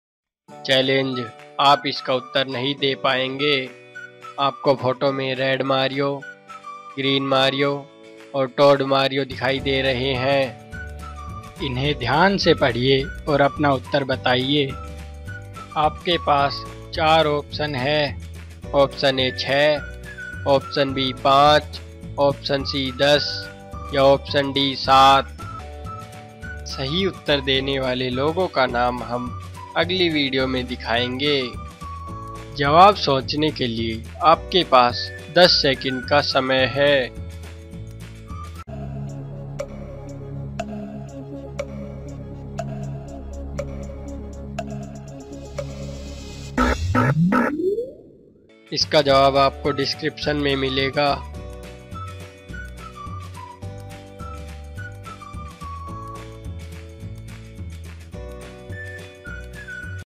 गणित की फोटो वाली पहेलियाँ | Mario Character Puzzle Paheli | मारियो गेम वाली पहेली Mario🤔🤔🤔
गणित की फोटो वाली पहेलियाँ | Mario Character Puzzle Paheli | मारियो गेम वाली पहेली

हैलो दोस्तो,
 यह वीडियों बहुत ही रोमांचक हैं | तो वीडियों को अंत तक जरूर देखिएगा | वीडियों देखने के लिए धन्यवाद |

इस पहेली का उत्तर इस प्रकार होगा –
यहाँ पर Red Mario = 10
Green Mario = 6
और है तो Tod Mario = 5

25 = 10 + 5 + 10
6 + 1 + 6 = 13
7 = 10 – 6  + 3
Tod Mario = 5

इस प्रकार इसका सही उत्तर (B) 5 आएगा |